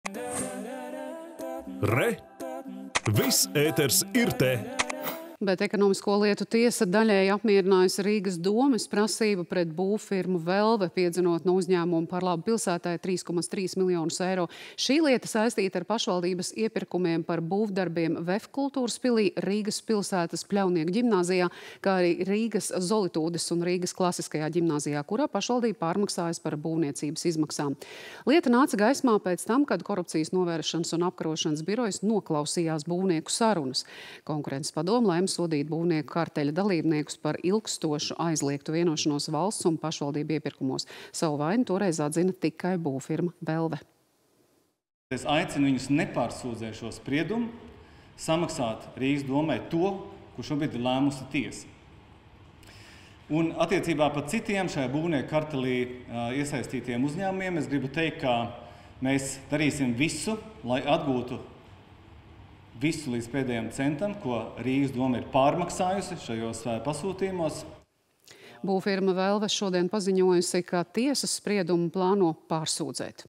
Rīgas dome no “Velves” piedzen 3,3 miljonus eiro
Ekonomisko lietu tiesa daļēji apmierinājusi Rīgas domes prasību pret būvfirmu “Velve”, piedzenot no uzņēmuma par labu pilsētai 3,3 miljonus eiro. Šī lieta saistīta ar pašvaldības iepirkumiem par būvdarbiem VEF Kultūras pilī, Rīgas pilsētas Pļavnieku ģimnāzijā, kā arī Rīgas Zolitūdes un Rīgas Klasiskajā ģimnāzijā, kurā pašvaldība pārmaksājusi par būvniecības izmaksām. Lieta nāca gaismā, pēc tam kad Korupcijas novēršanas un apkarošanas birojs noklausījās būvnieku sarunas. Konkurences padome lēma sodīt būvnieku karteļa dalībniekus par ilgstošu aizliegtu vienošanos valsts un pašvaldību iepirkumos. Savu vainu toreiz atzina tikai būvfirma “Velve”.